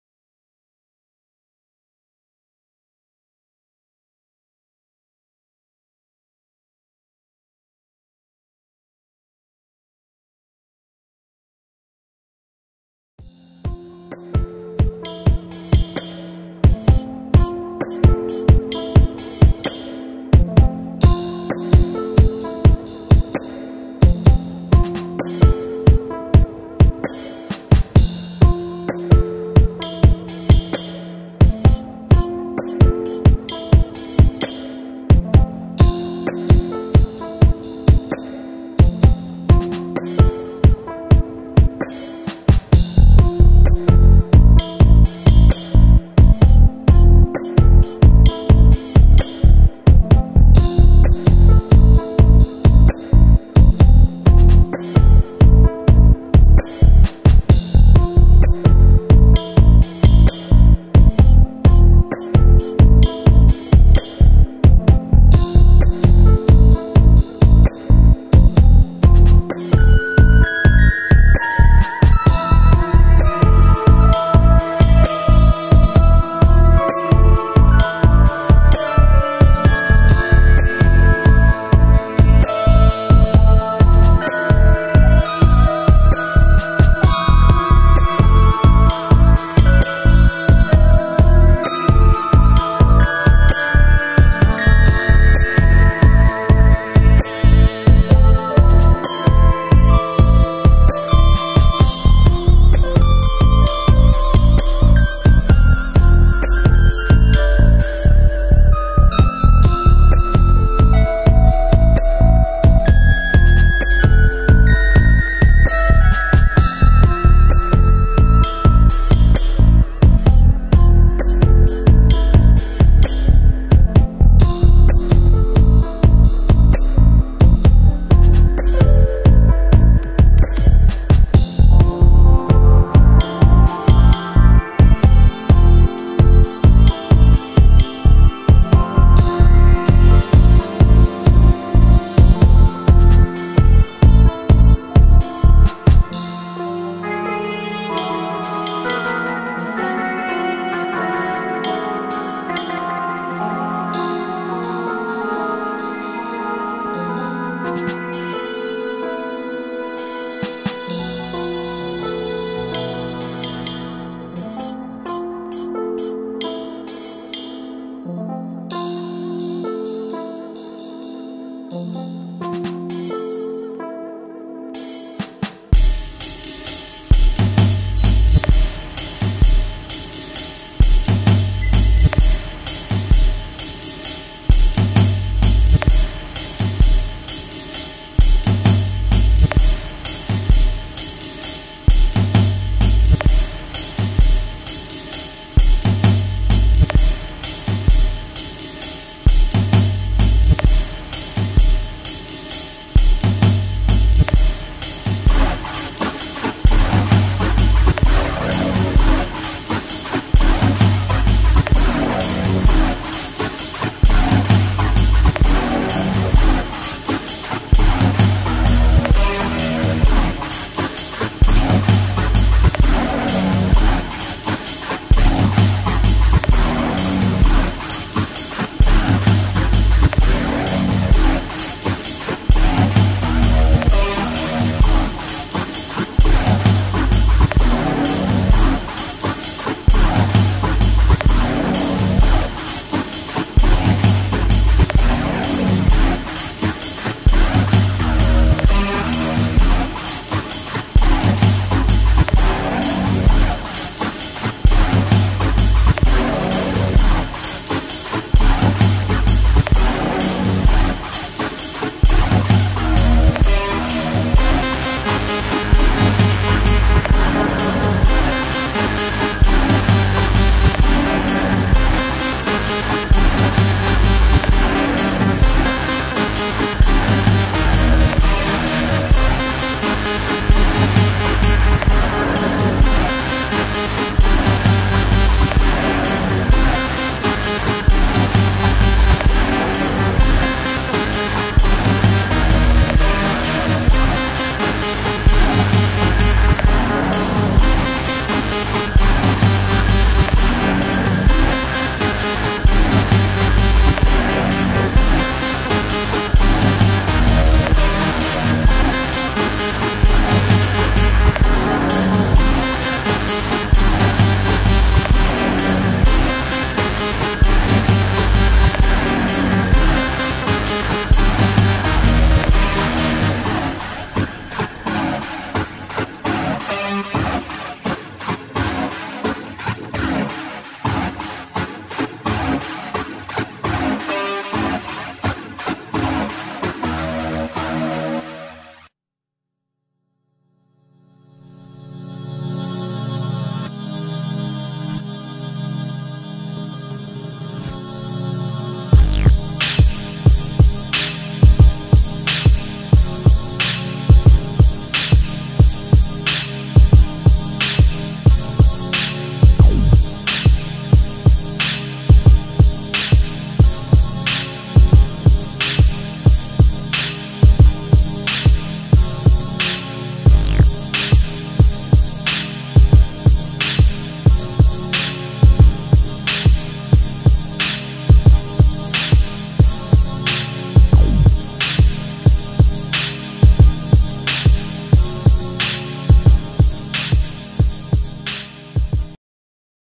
First Born Art // Abstract Painting Time Lapse
47 minutes into 6 minutes